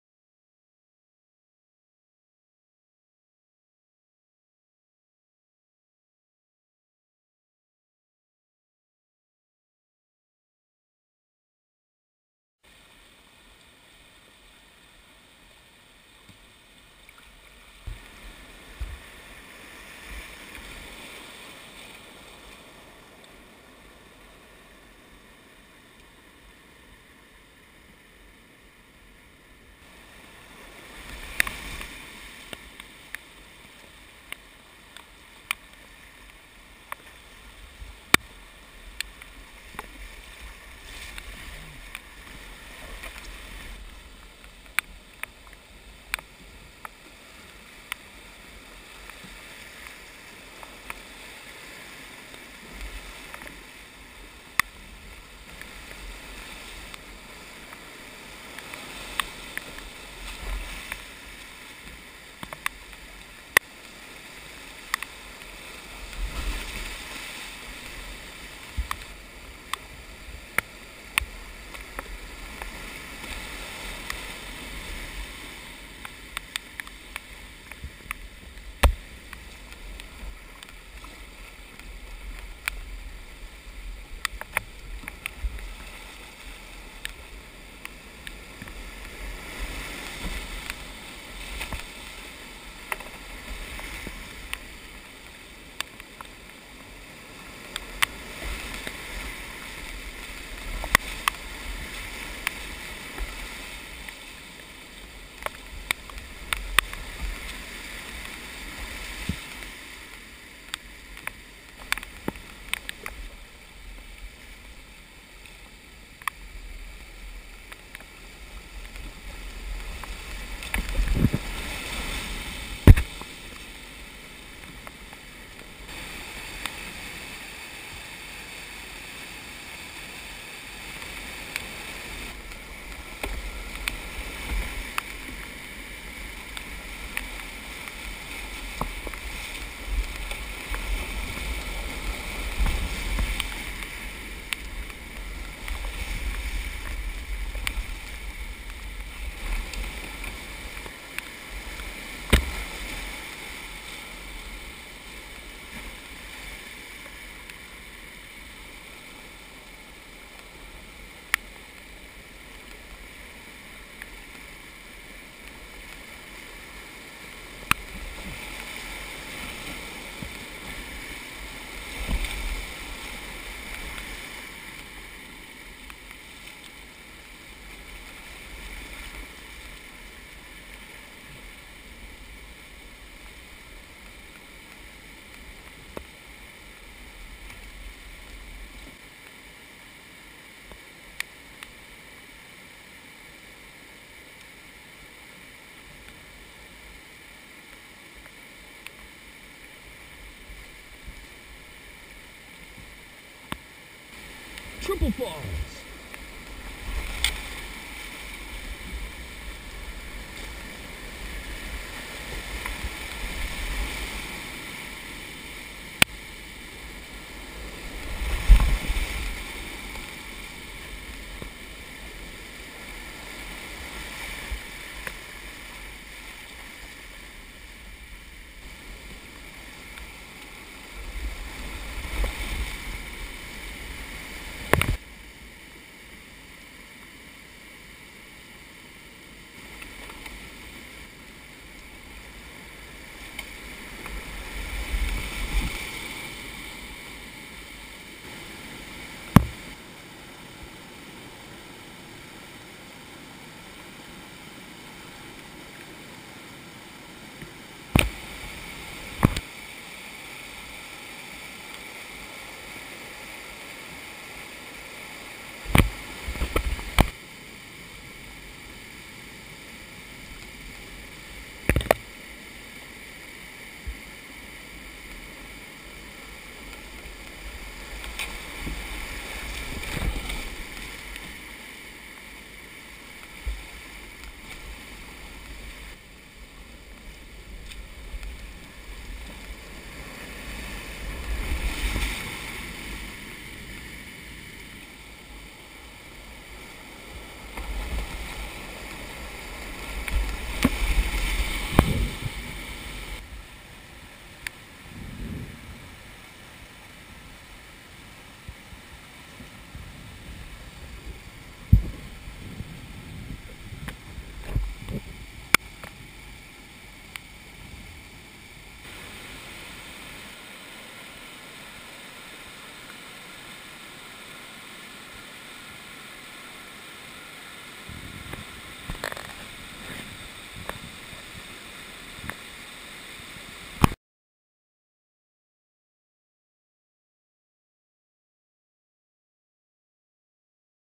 A helmet cam view of the Upper Tallulah
Canoe and kayak runs on the upper Tallulah in Georgia.  The water is on the low side but we had fun.